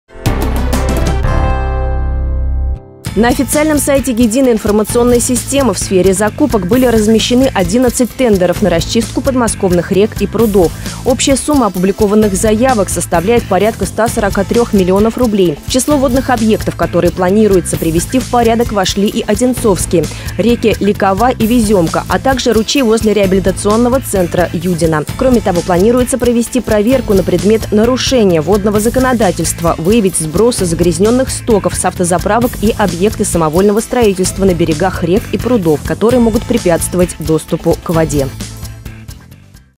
На расчистку прудов и рек в Подмосковье выделят около 143 млн рублей
На официальном сайте Единой информационной системы в сфере закупок были размещены 11 тендеров на расчистку подмосковных рек и прудов. Общая сумма опубликованных заявок составляет порядка 143 миллионов рублей. В число водных объектов, которые планируется привести в порядок, вошли и одинцовские.